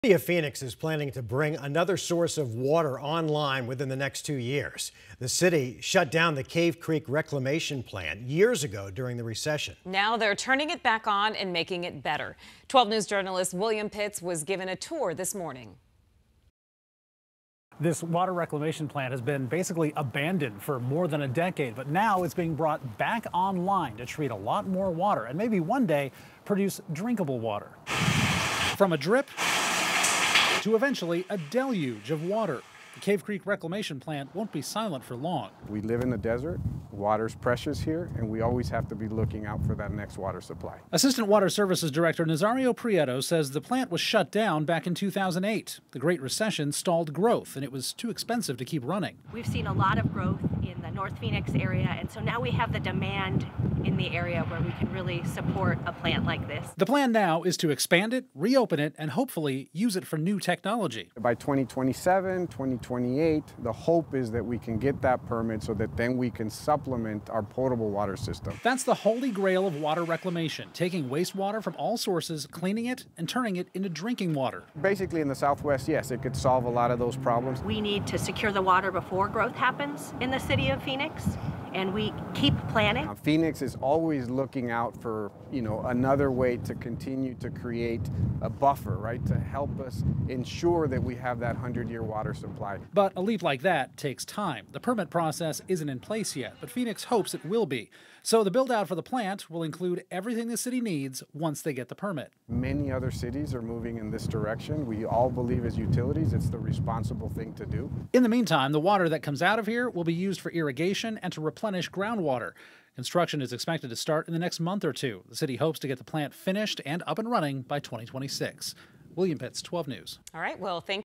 Waste water to drinking water? Reclamation plant makes a comeback in Phoenix
City officials say reopening the Cave Creek plant could create a valuable new water resource for Phoenix.